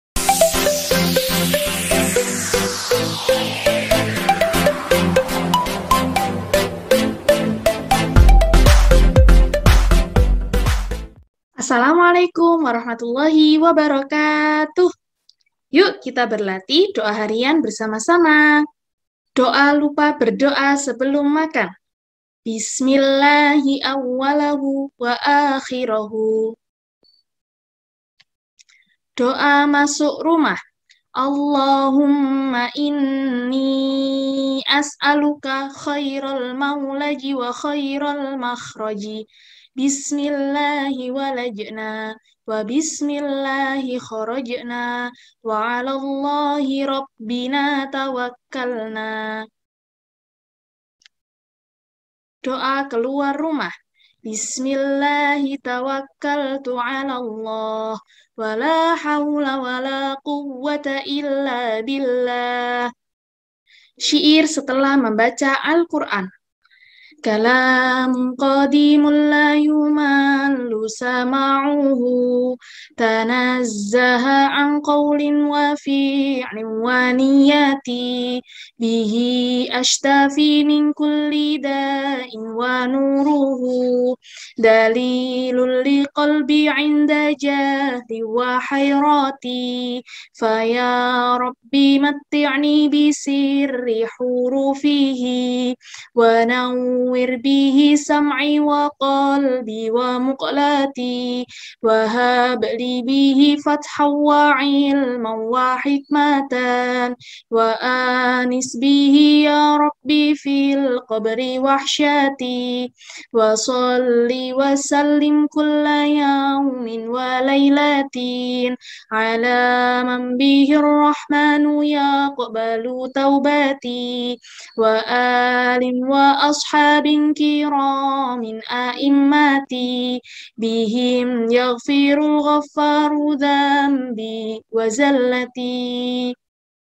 Doa Harian Bagian 2 - Kelas 3
Video Doa Lupa Berdoa Sebelum Makan, Masuk Rumah, Keluar Rumah, dan Syi'ir Membaca Al Qur'an